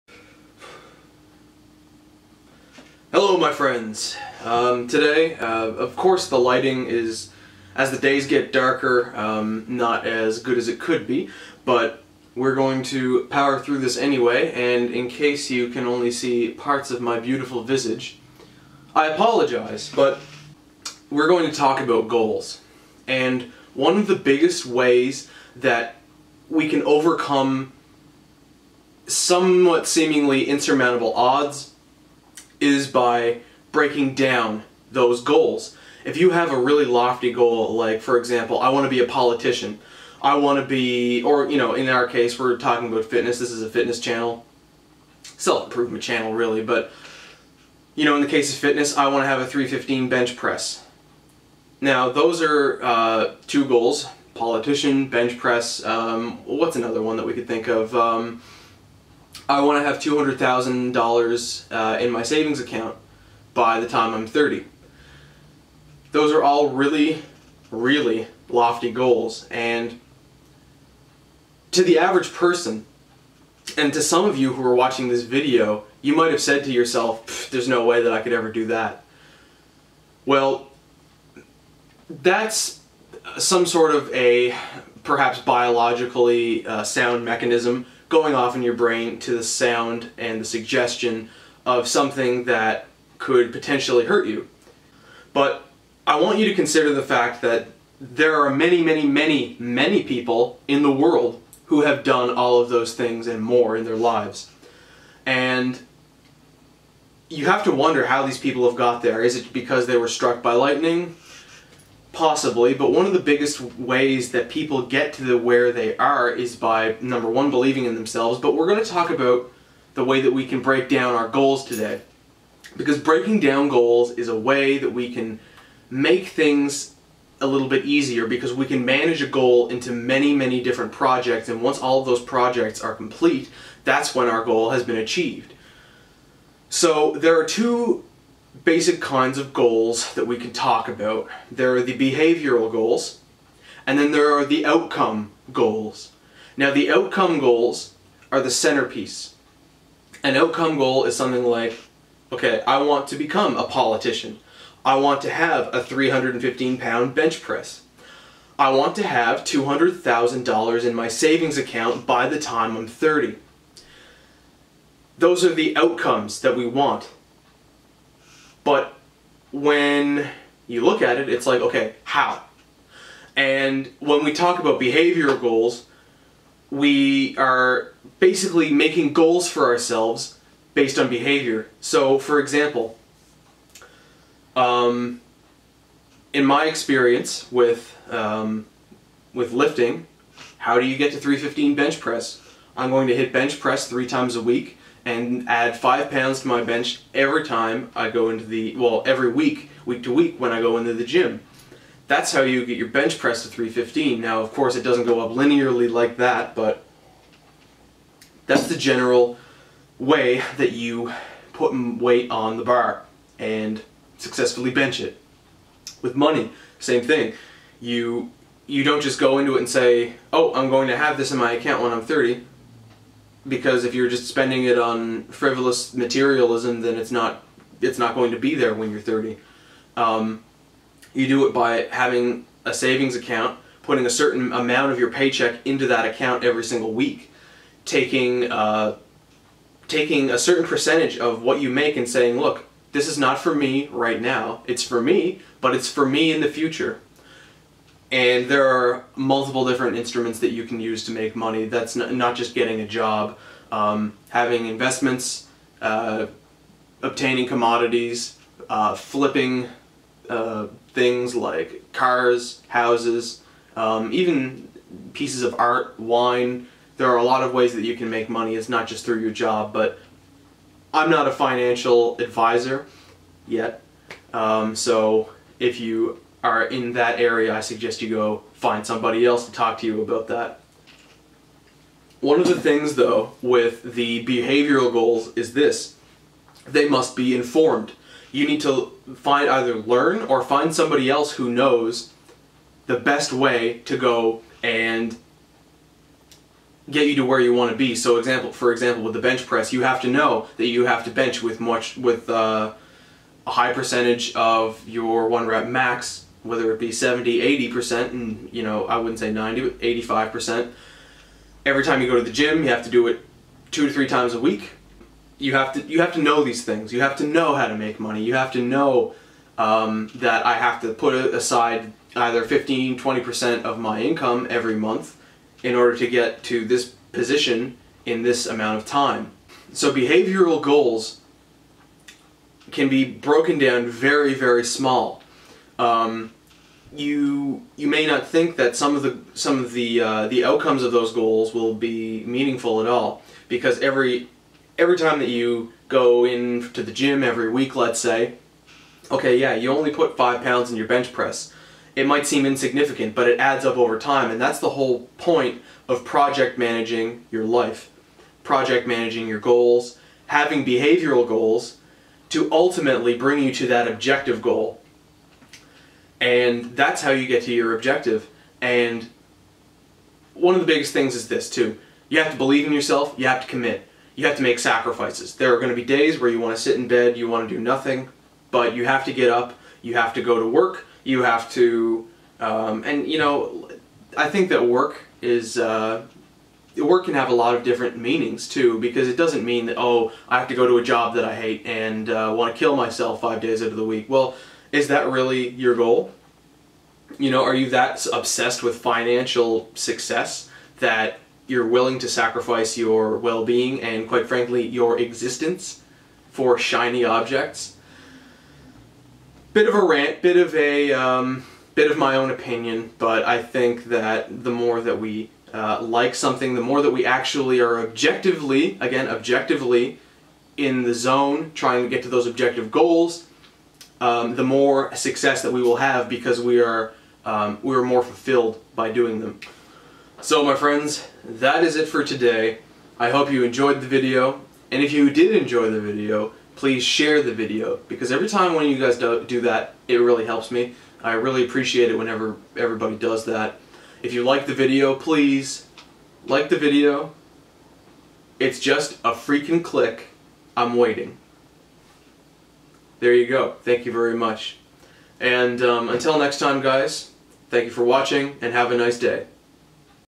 Managing Your Goals Through Behavioral Goals and Projects
A quick video about project managing your goals and breaking your big scary goals into small behaviors and habits that you can achieve easily each day.